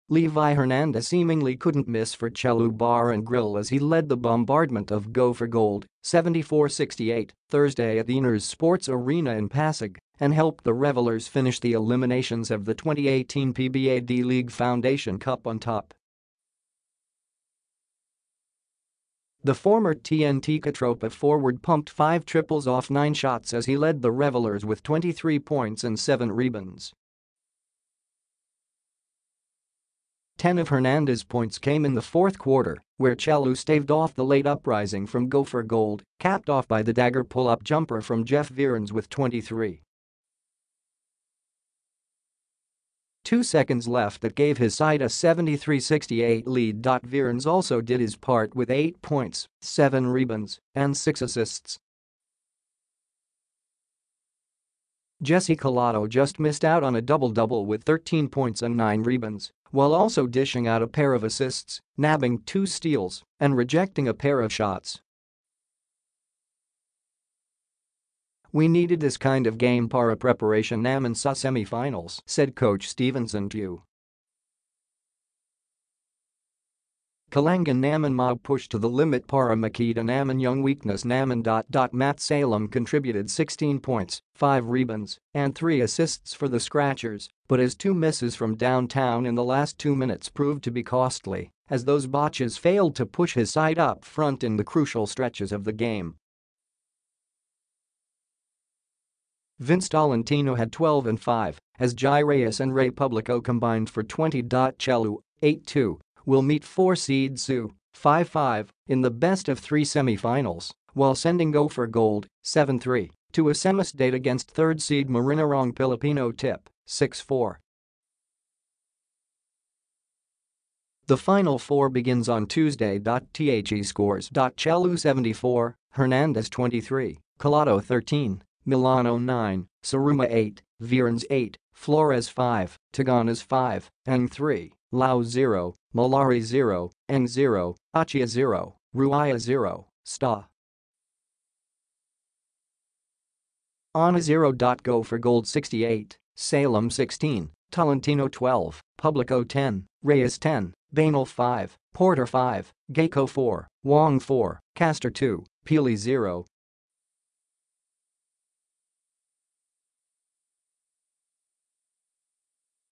Levi Hernandez tows Che'Lu to top seed
Levi Hernandez seemingly couldn’t miss for Che’Lu Bar and Grill as he led the bombardment of Go for Gold, 74-68, Thursday at Ynares Sports Arena in Pasig and helped the Revellers finish the eliminations of the 2018 PBA D-League Foundation Cup on top.

The former TNT KaTropa forward pumped five triples off nine shots as he led the Revellers with 23 points and seven rebounds.

Ten of Hernandez’ points came in the fourth quarter – where Che’Lu staved off the late uprising from Go for Gold – capped off by the dagger pull-up jumper from Jeff Viernes with 23.2 seconds left that gave his side a 73-68 lead.

Viernes also did his part with eight points, seven rebounds, and six assists.

Jessie Collado just missed out on a double-double with 13 points and nine rebounds, while also dishing out a pair of assists, nabbing two steals, and rejecting a pair of shots.

“We needed this kind of game para preparation namin sa semifinals,” said coach Stevenson Tiu. “Kailangan namin ma-push to the limit para makita namin yung weakness namin.”

Matt Salem contributed 16 points, five rebounds, and three assists for the Scratchers, but his two misses from downtown in the last two minutes proved to be costly, as those botches failed to push his side up front in the crucial stretches of the game.

Vince Tolentino had 12 and five, as Jai Reyes and Rey Publico combined for 20.

Che’Lu (8-2) will meet four-seed CEU (5-5) in the best-of-three semifinals, while sending Go for Gold (7-3) to a semis date against third seed Marinerong Pilipino-TIP (6-4). The Final Four begins on Tuesday.

The Scores:

Che’Lu 74 – Hernandez 23, Collado 13, Melano 9, Siruma 8, Viernes 8, Flores 5, Taganas 5, Ng 3, Lao 0, Mallari 0, Ng 0, Ochea 0, Ruaya 0, Sta. Ana 0.

Go for Gold 68 – Salem 16, Tolentino 12, Publico 10, Reyes 10, Banal 5, Porter 5, Gaco 4, Wong 4, Castor 2, Pili 0.

Quarterscores: 13-16, 31-31, 56-50, 74-68.